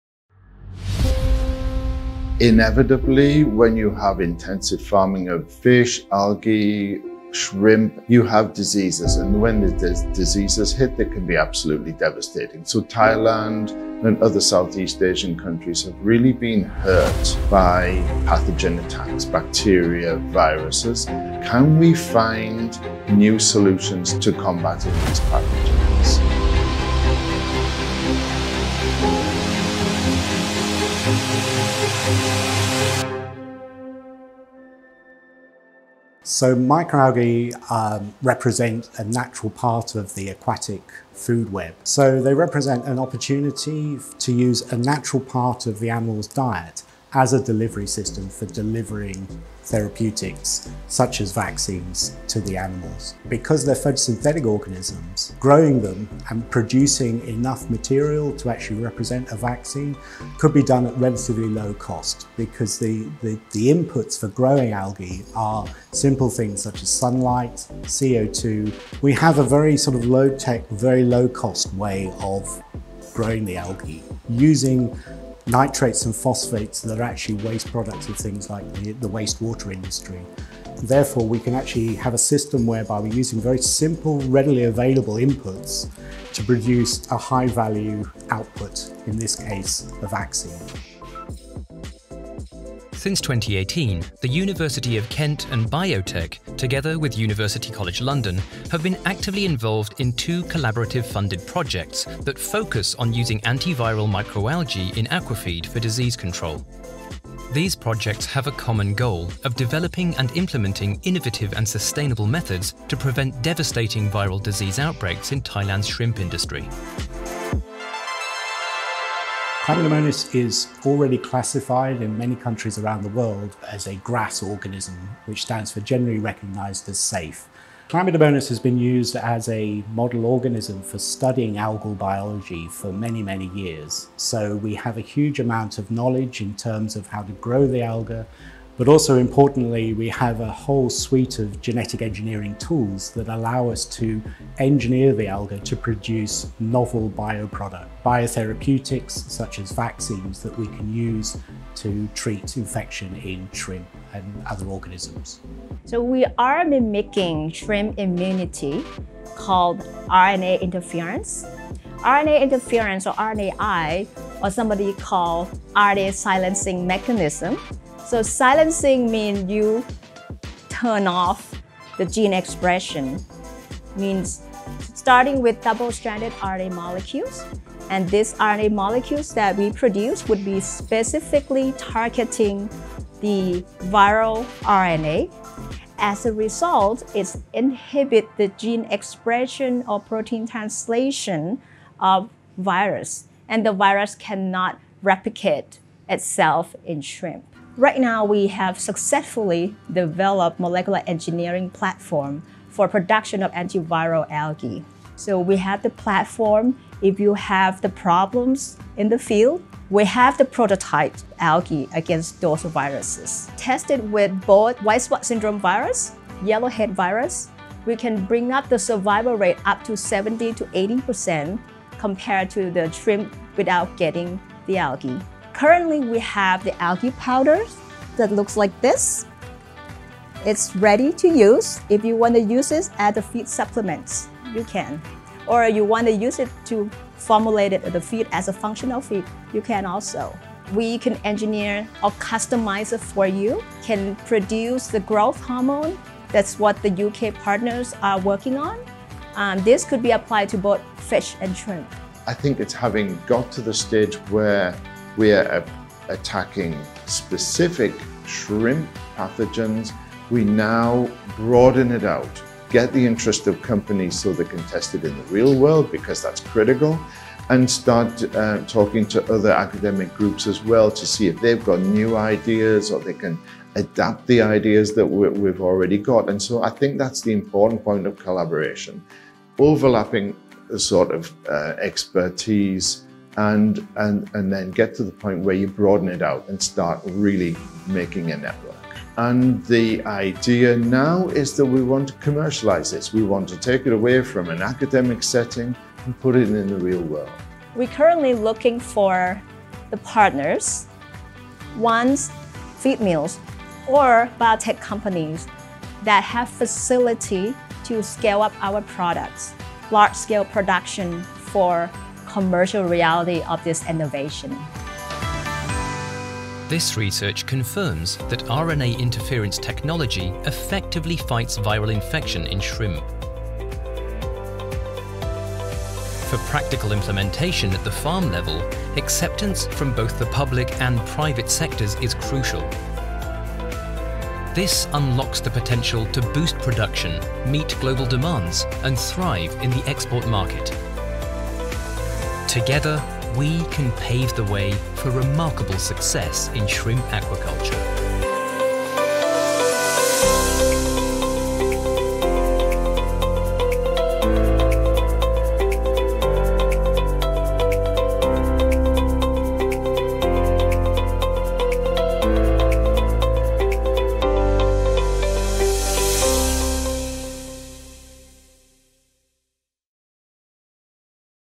Algal Biotechnology in Shrimp Aquaculture
ข้อมูลภาษาไทย สามารถติดตามได้ที่

Shrimp aquaculture has been threatened by significant losses due to viral pathogens. Methods of disease control are still impractical and expensive. @UniversityofKent and @BIOTECNSTDA in collaboration with @UCLTV  have been actively involved in two collaborative funded projects that focus on using antiviral microalgae in aquafeed for disease control.